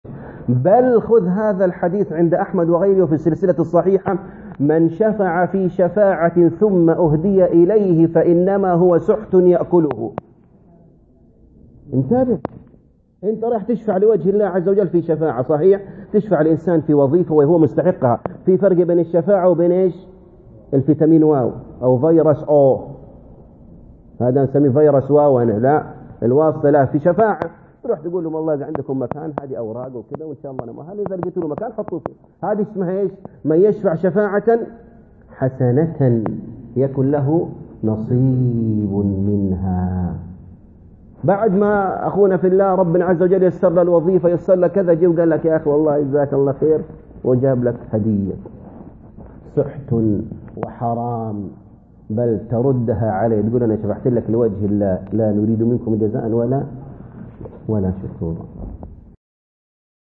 أخذ الهدية والأجرة على الشفاعة . من شفع ثم أهدي إليه . الشيخ النميري
١٤٤٠/٢/٢
من شفع في شفاعة ثم أُهديَ إليه فإنما هو سحت يأكله ..الشيخ النميري حفظه الله